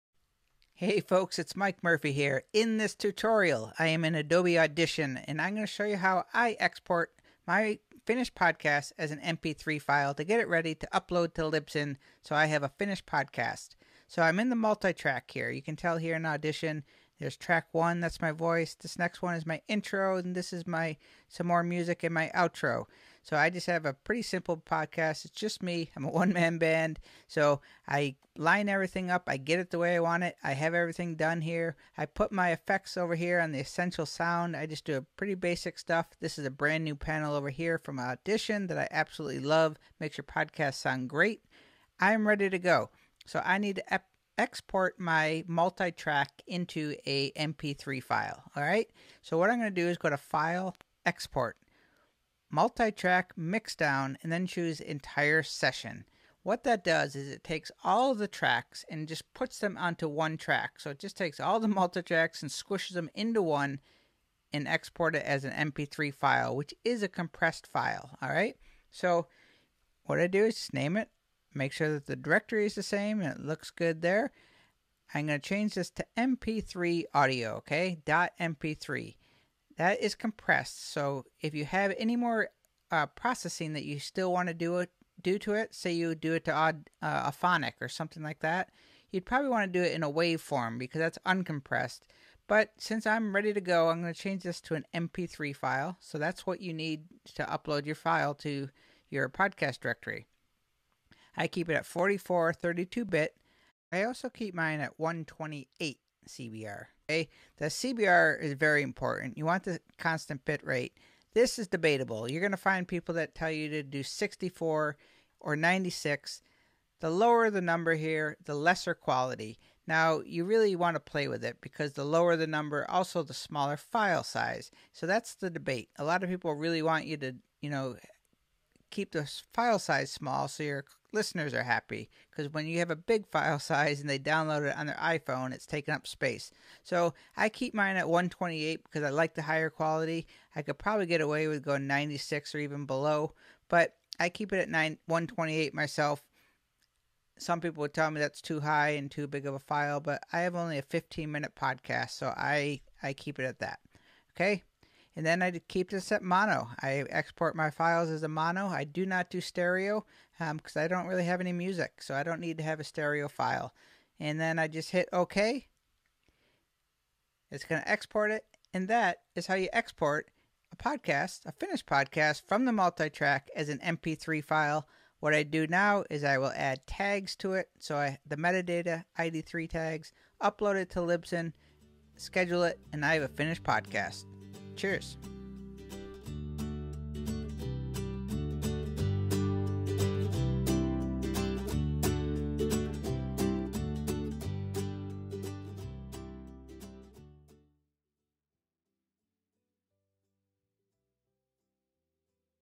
How To Export Podcast As .mp3 file in Adobe Audition
How To Export Podcast from  Multitrack Session To .mp3 file .

File Export
Multitrack Mixdown
Entire Session
44.1,  Mono, 32-Bit Float
128 kbps, CBR